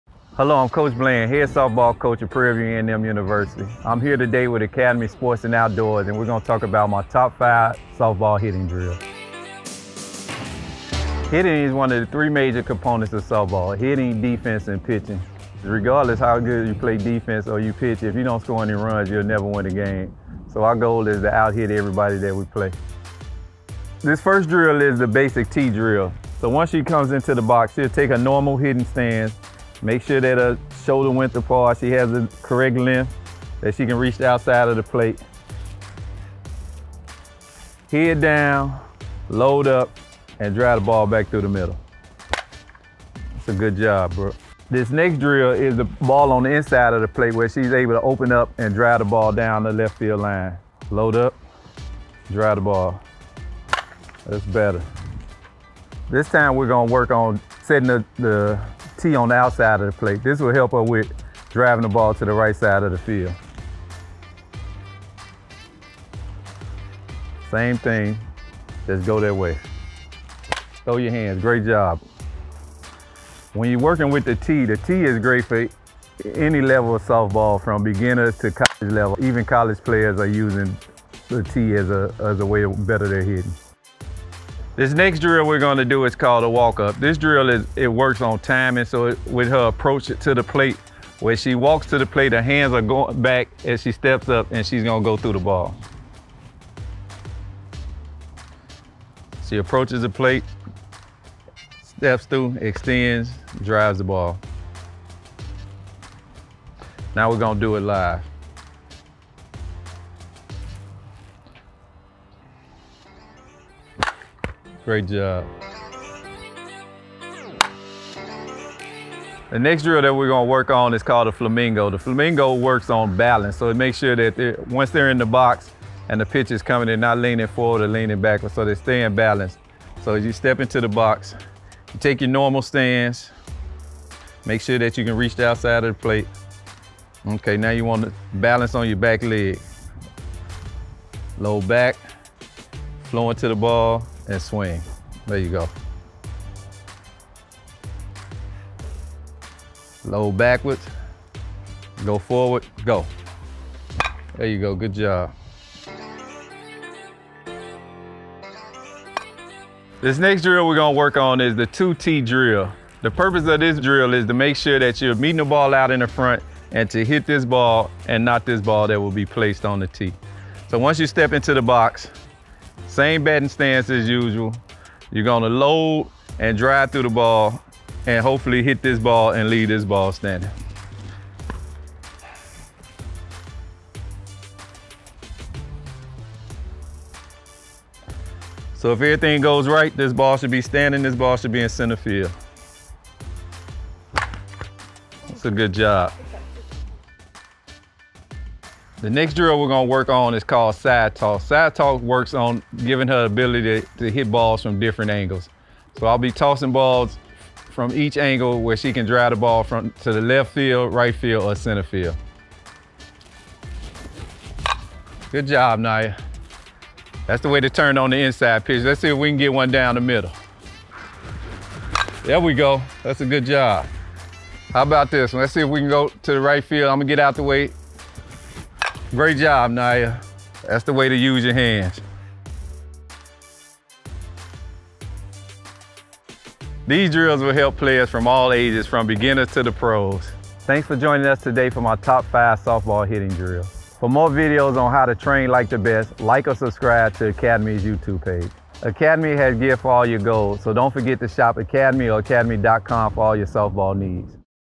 Softball | Hitting Drills with Coach Bland
Prairie View A&M University head softball coach Vernon Bland runs through his top 5 hitting drills for all levels of play. Find new ways to perfect your swing this season.

1) 0:29 Basic Tee Drills
2) 1:23 Walk Up Drill
3) 1:59 The Flamingo
4) 2:39 Two Tee Drill
5) 3:18 Side Toss